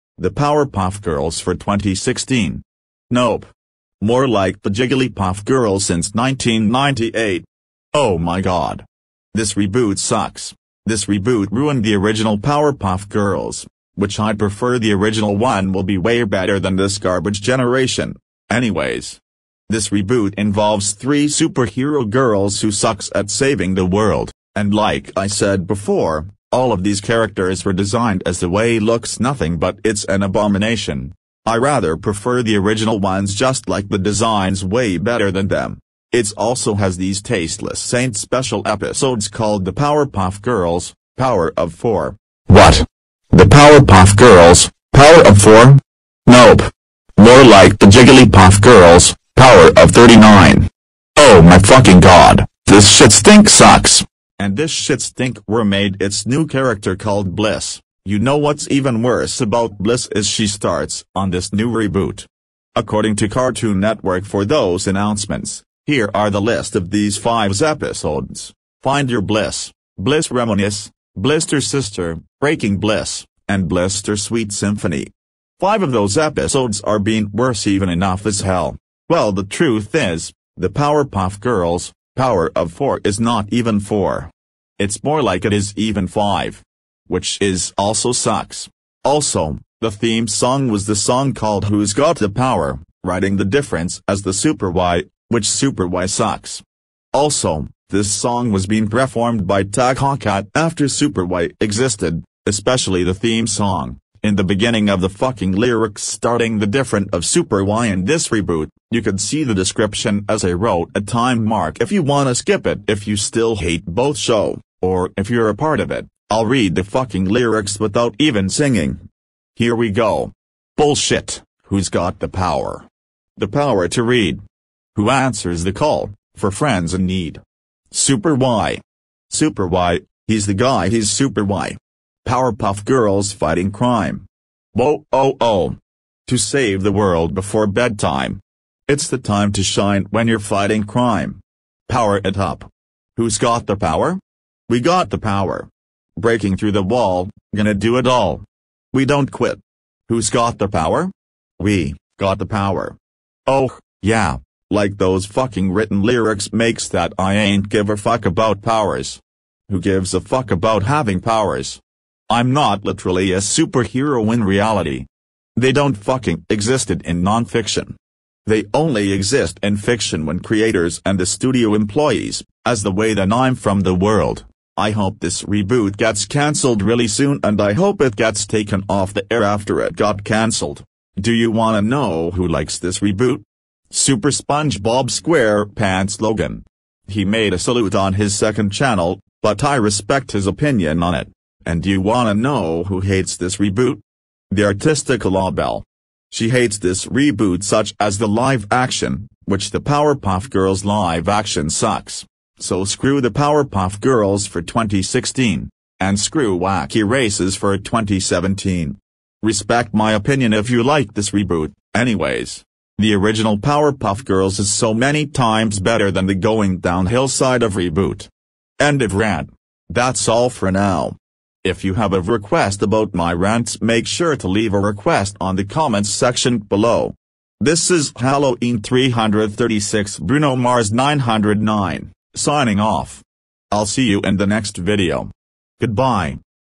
My Rants Show Season 2 Episode 2: The Powerpuff Girls (2016)
credits goes to respecitve owners 2:34

what the heck are those powerpuff girls series creators are thinking